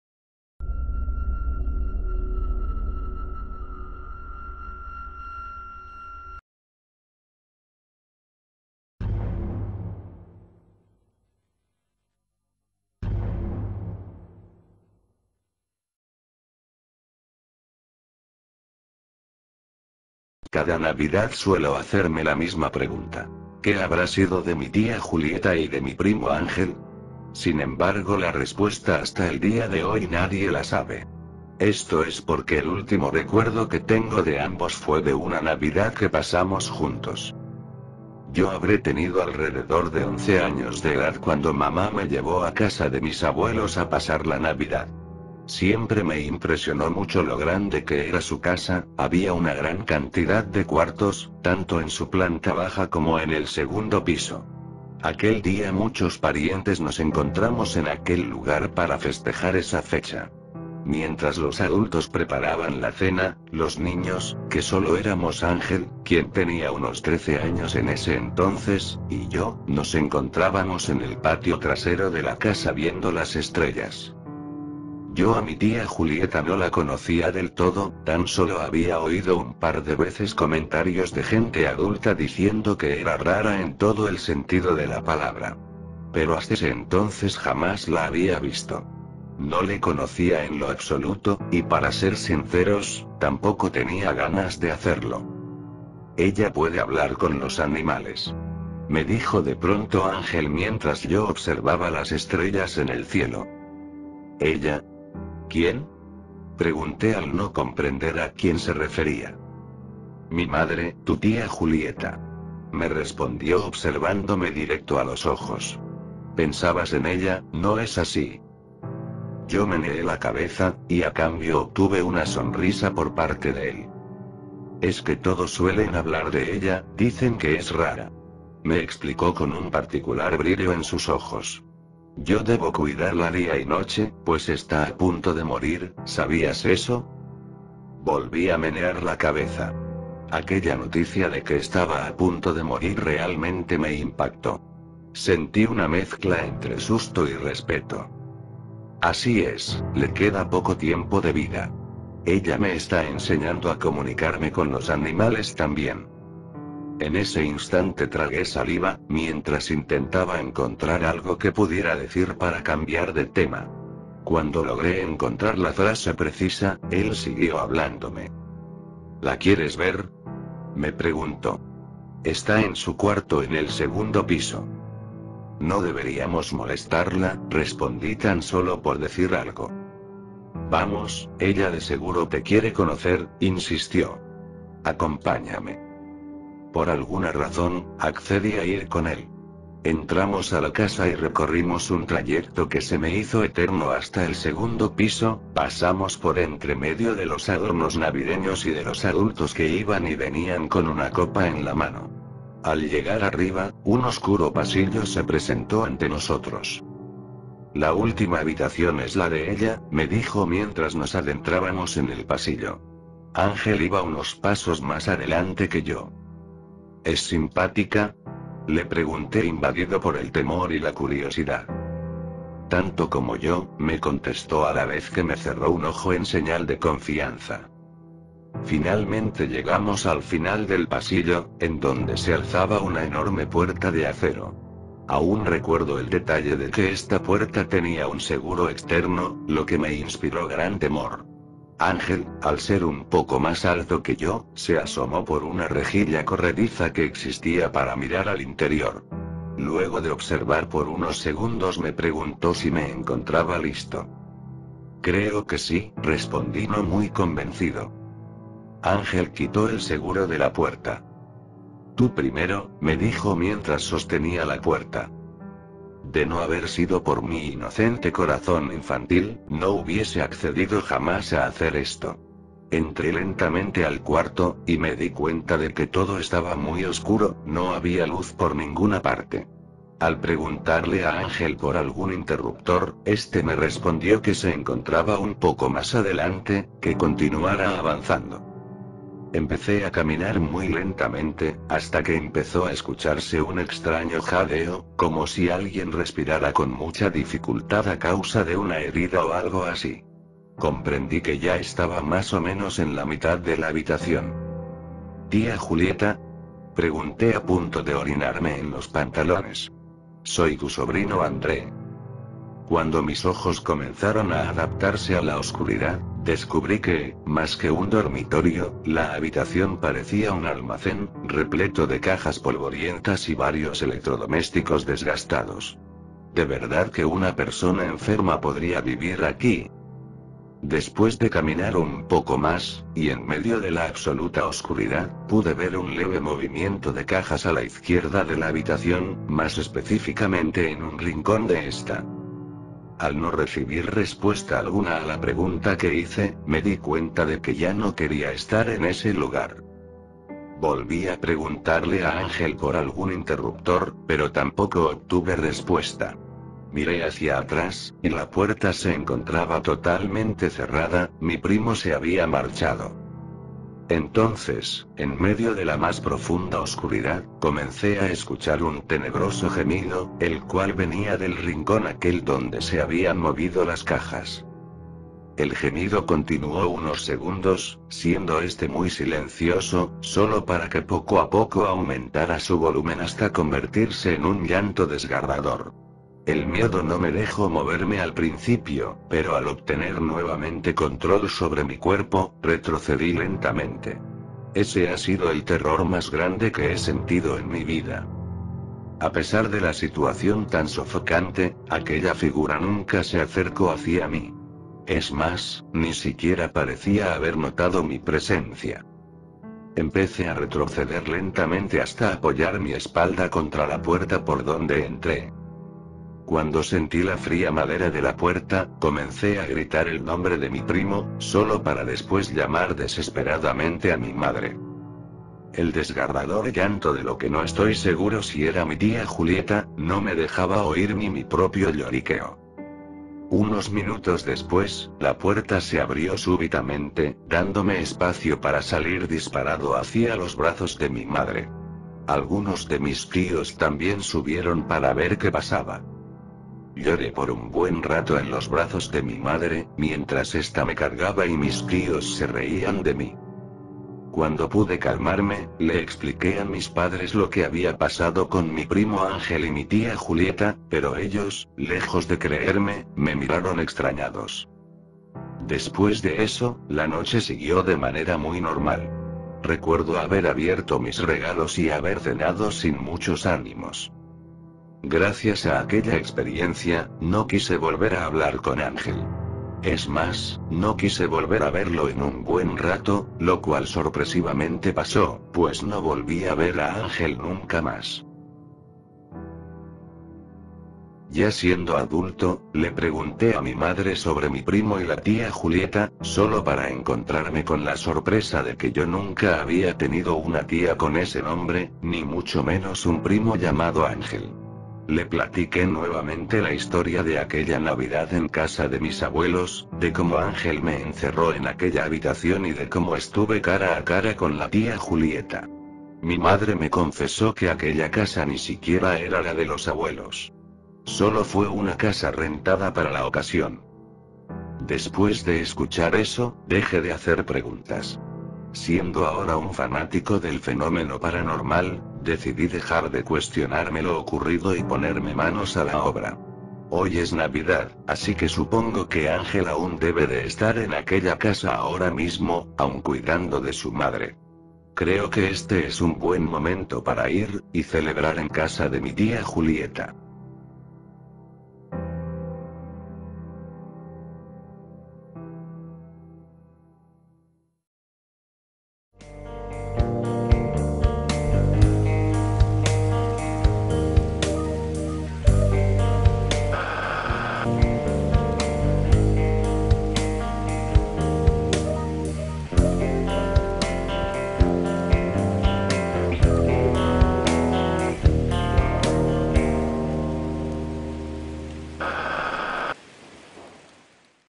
Navidad en casa de la tia Julieta | Creepypasta [Daniel Cross]
¡Especial de navidad 2022!

Esta historia a sido adaptada a un formato actualizado. El texto es estrictamente el mismo, agregando pequeños elementos y/o detalles para obtener un mejor entendimiento y disfrute de la narración.

La pieza musical del Outro es propiedad del canal WYS (Aunque actualmente parece estar fuera de servicio):